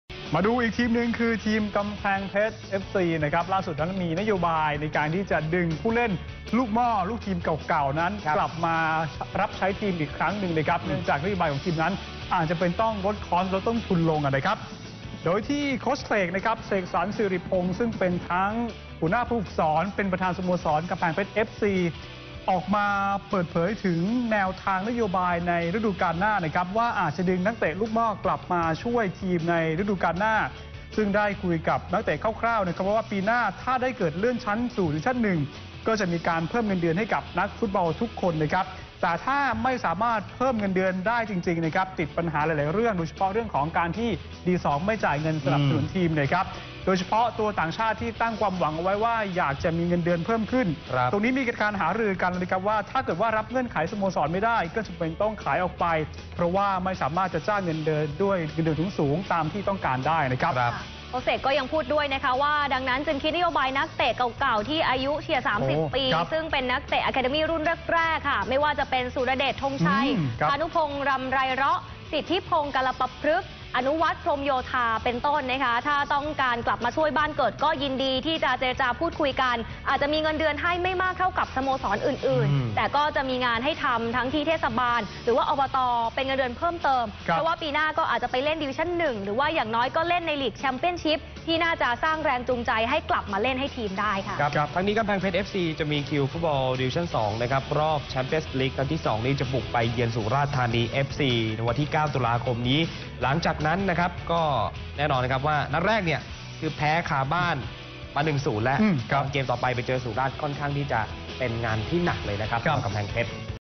"กำแพงเพชร" ผุดนโยบายดึงนักเตะลูกหม้อช่วยทีม
พิกัดกีฬา ช่วงเที่ยง : ทีมกำแพงเพชร เอฟซี มีคิวในศึกฟุตบอลดิวิชั่นสองรอบ แชมเปี้ยนส์ลีก นัดที่สองโดยจะต้องบุกไปเยือน สุราษฏ์ธานี เอฟซี วันอาทิตย์ที่ 9 ตุลาคมนี้ หลังจากนัดแรกแพ้คาบ้านต่อทีมแชมป์จากภาคใต้มา 1-0 "NOW26" 6-10-59